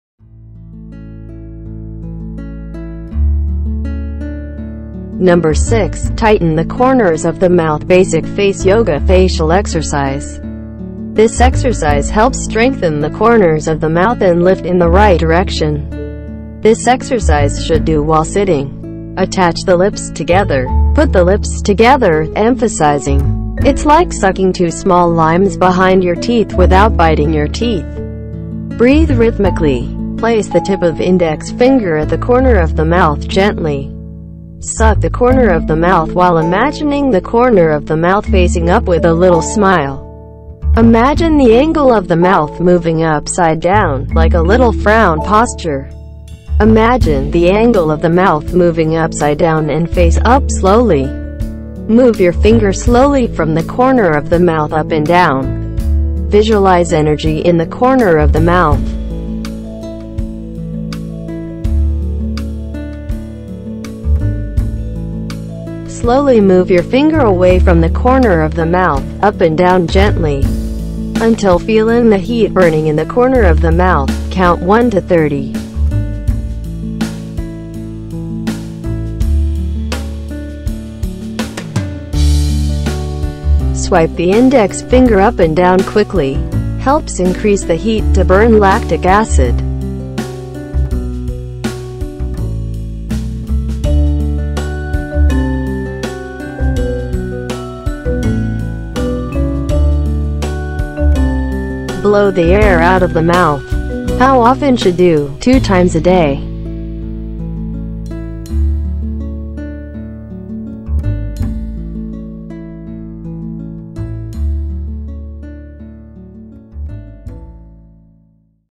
NO.6: TIGHTEN THE CORNERS OF THE MOUTH BASIC FACE YOGA FACIAL EXERCISE โยคะหน้ายกกระชับมุมปาก
👩🏻‍🦰 Find Out Your Skin Priority and Free Secret Natural Beauty Tips You Need to Know

🤩ฟรี สนใจมาเข้าร่วมแก้ปัญหานี้ ฟรี🤩
📌พูดคุยกับพี่มิ้มได้ที่ Line OA: @mimpfitfaceyoga

NO.6: TIGHTEN THE CORNERS OF THE MOUTH BASIC FACE YOGA FACIAL EXERCISE

BENEFITS:
This exercise helps strengthen the corners of the mouth and lift in the right direction.

HOW TO:
1. This exercise should do while sitting.
2. Attach the lips together. Put the lips together, emphasizing. It's like sucking 2 small limes behind your teeth without biting your teeth.
3. Breathe rhythmically.
4. Place the tip of the index finger at the corner of the mouth gently. Suck the corner of the mouth while imagining the corner of the mouth facing up with a little smile.
5. Imagine the angle of the mouth moving upside down, like a little frown posture. Imagine the angle of the mouth moving upside down and face up slowly.
6. Move your finger slowly from the corner of the mouth up and down. Visualize energy in the corner of the mouth.
7. Slowly move your finger away from the corner of the mouth, up and down gently. Until feeling the heat burning in the corner of the mouth, count 1 to 30.
8. Swipe the index finger up and down quickly. Helps increase the heat to burn lactic acid.
9.Blow the air out of the mouth.

HOW OFTEN SHOULD DO?: 2 times a day.